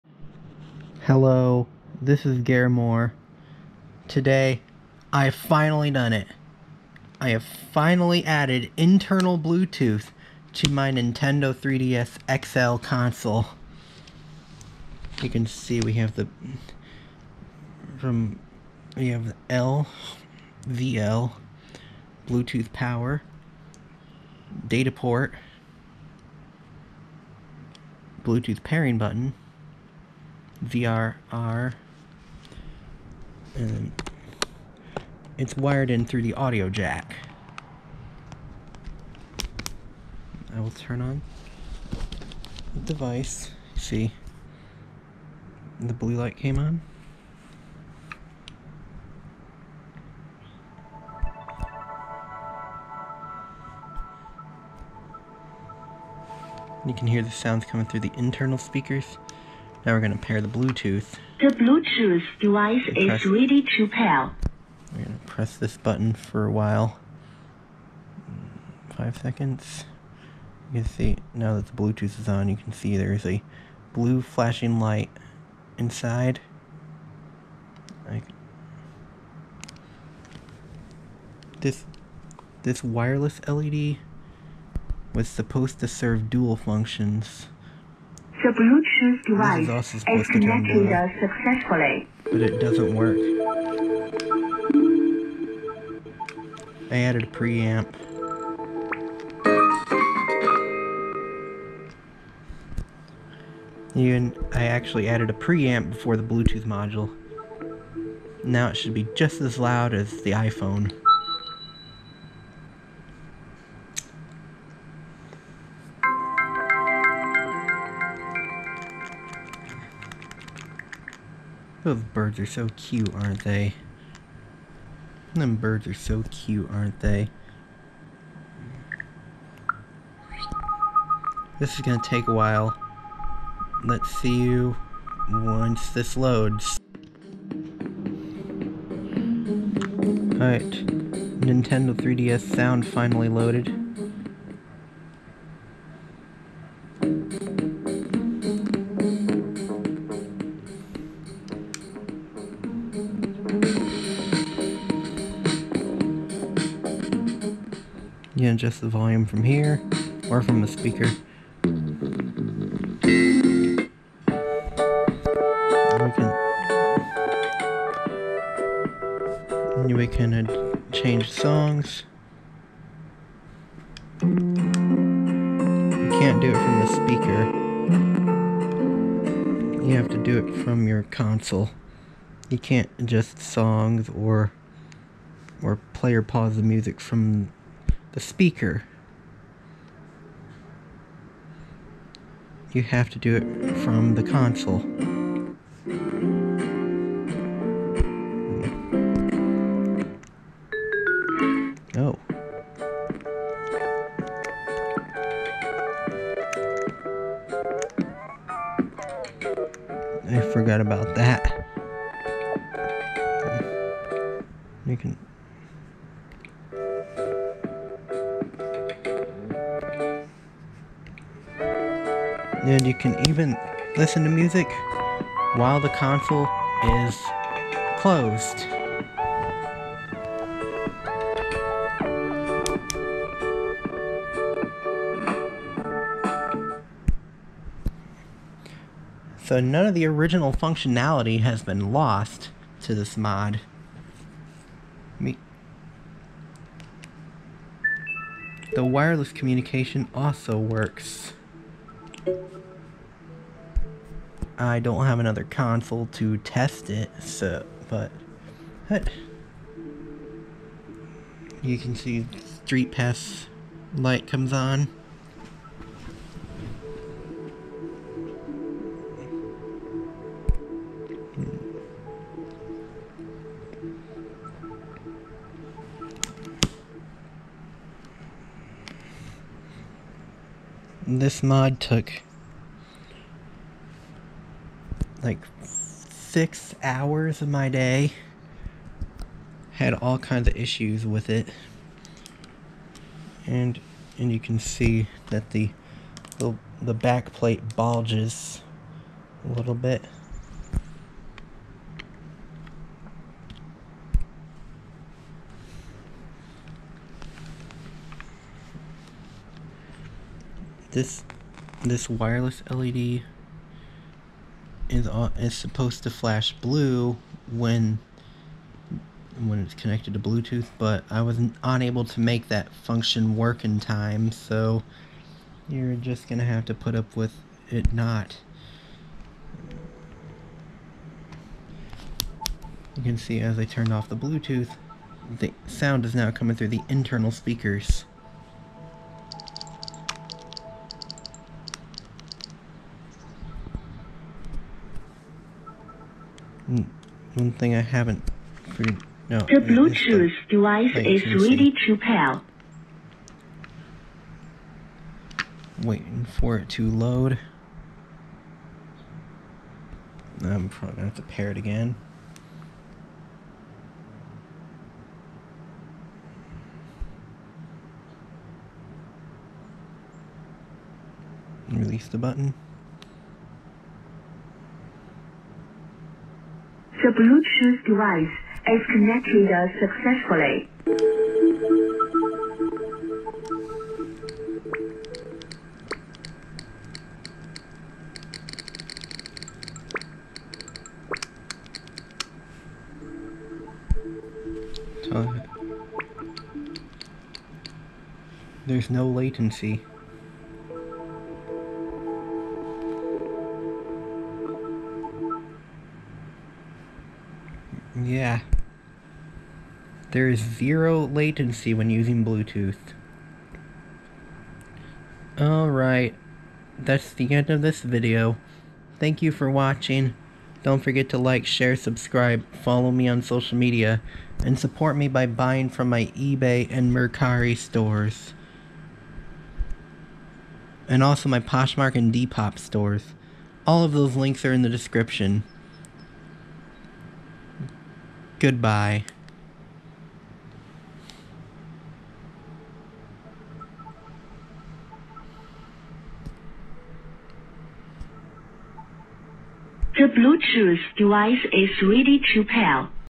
Nintendo 3DS Bluetooth Mod
I finally added internal Bluetooth to my Nintendo 3DS XL.

This mod took 6 hours to complete, there are some minor issues, but it works as expected with no detectable latency & no functionality loss.